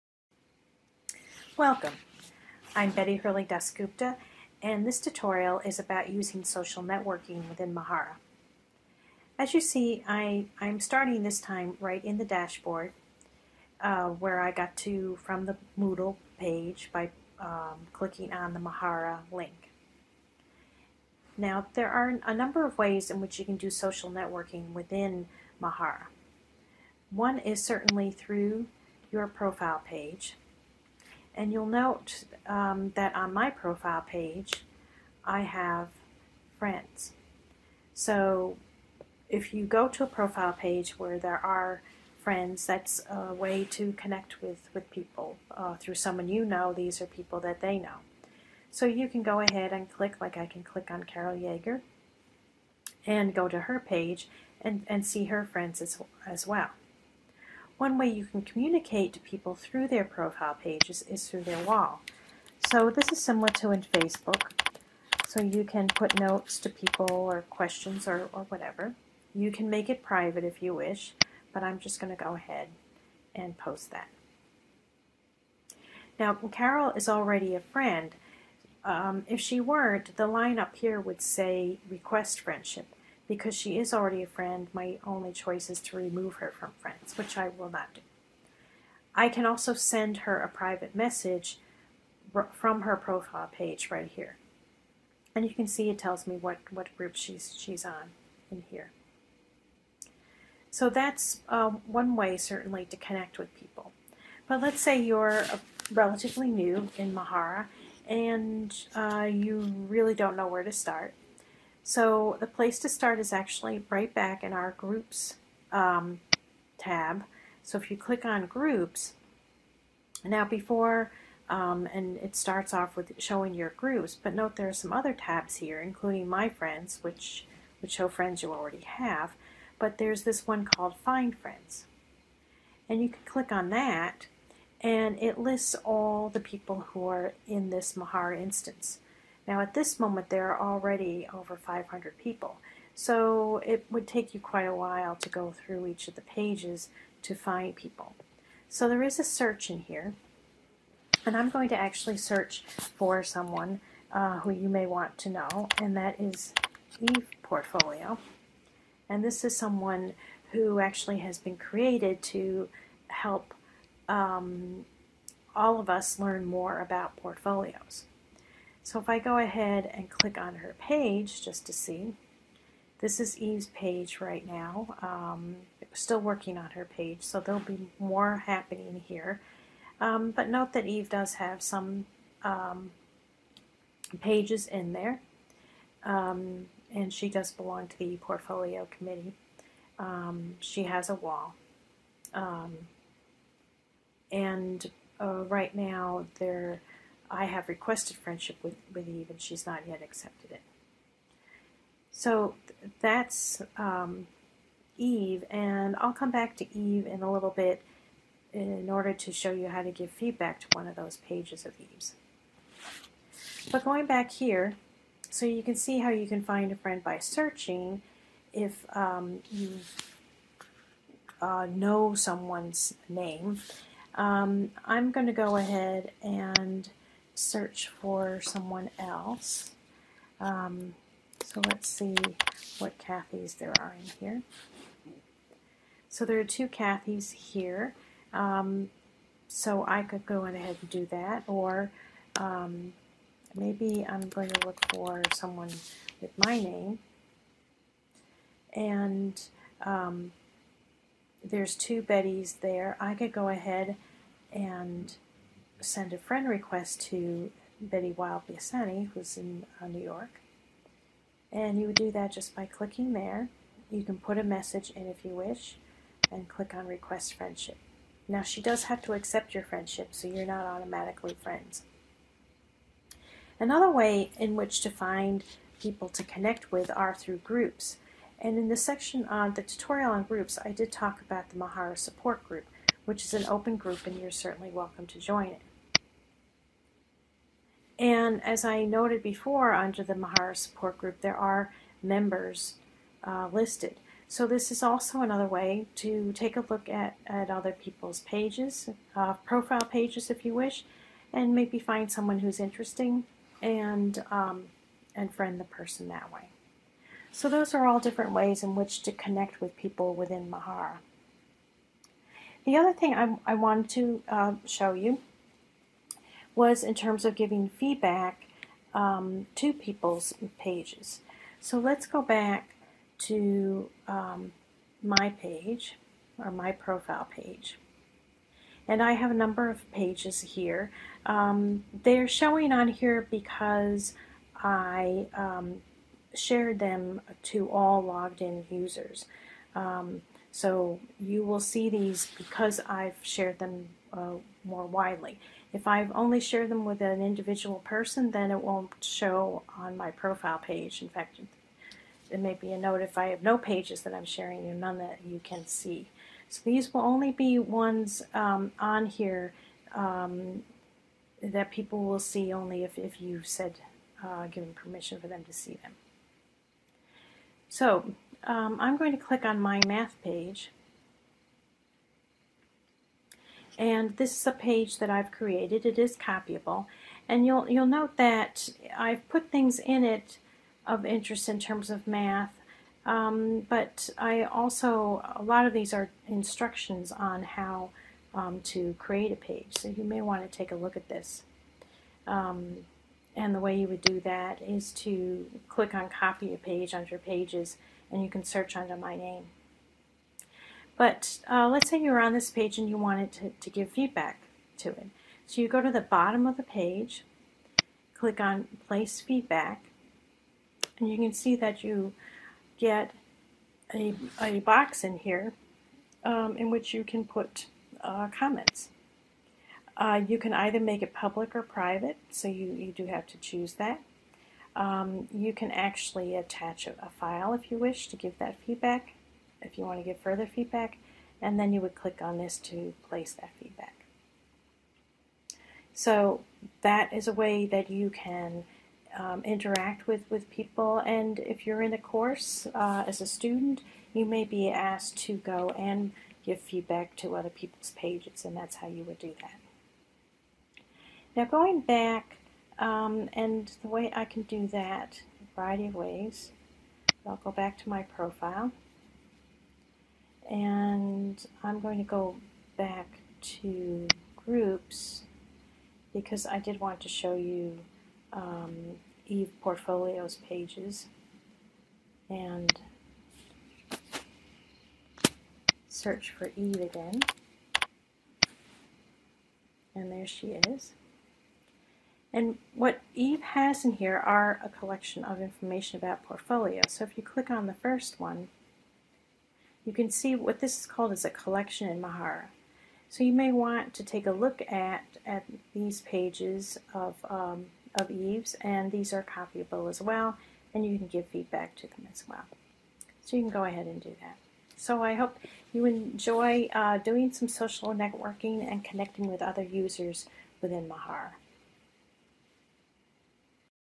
ESC Mahara Social Networking
finding and connecting with friends in Mahara